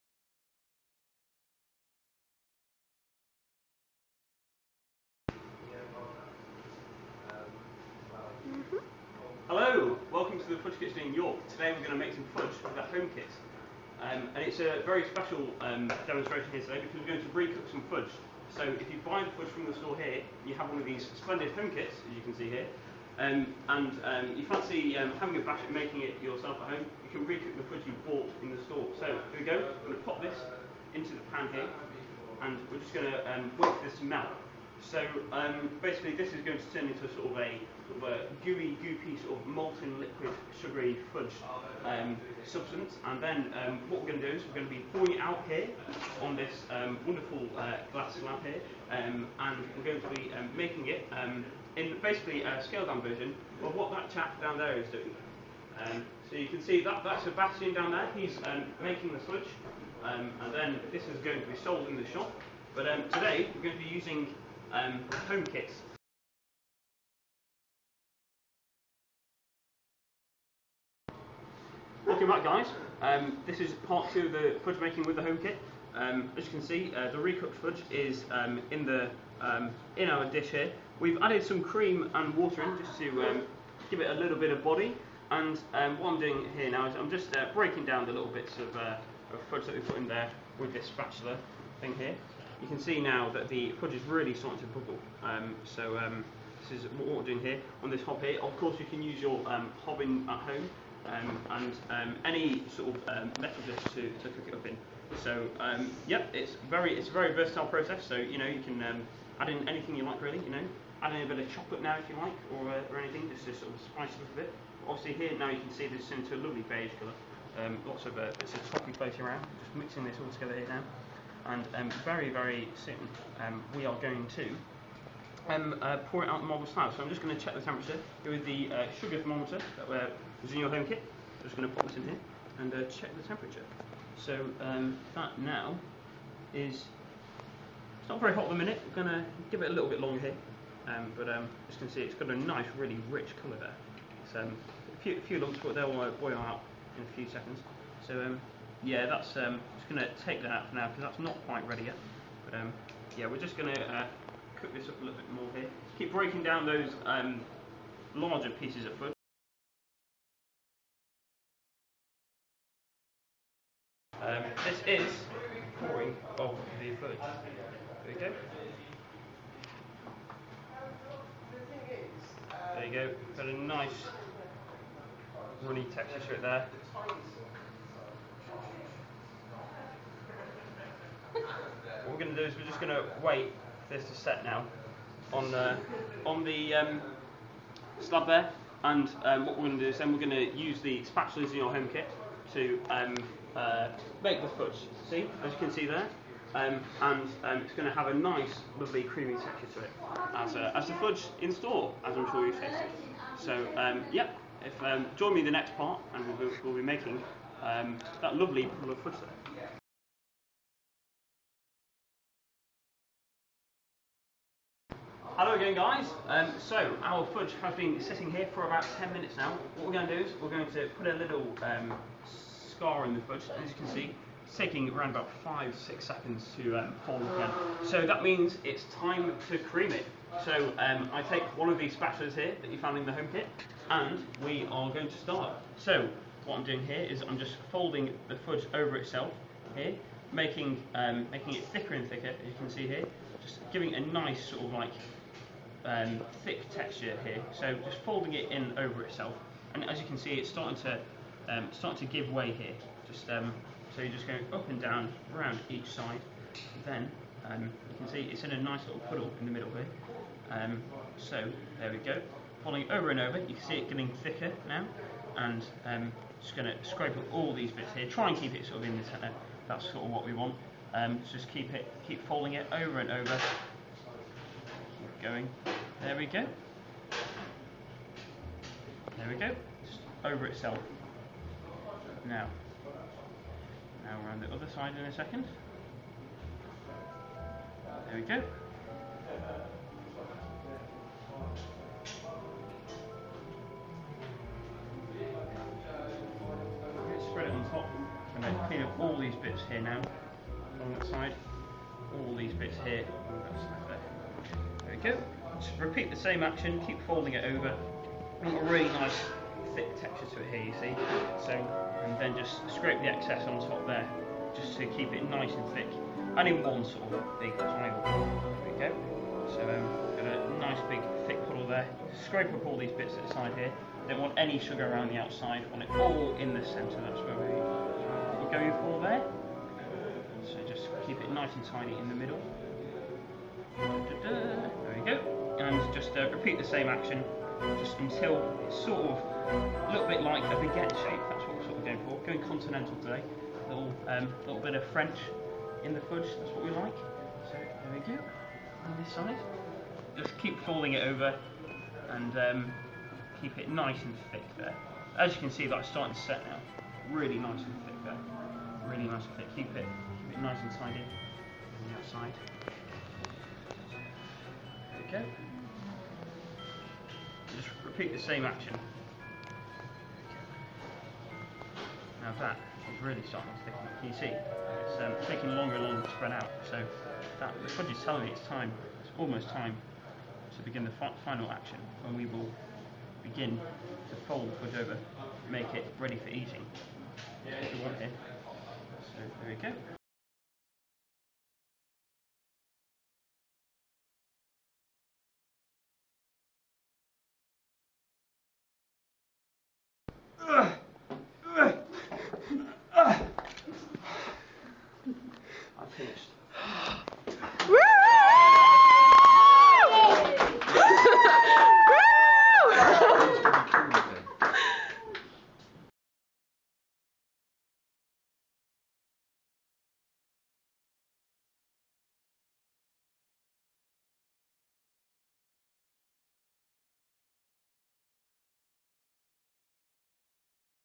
Fudge Kitchen York: Home Kit
Starring Ollie.
Ollie demonstrates how to use one of our fantastic Home Kits.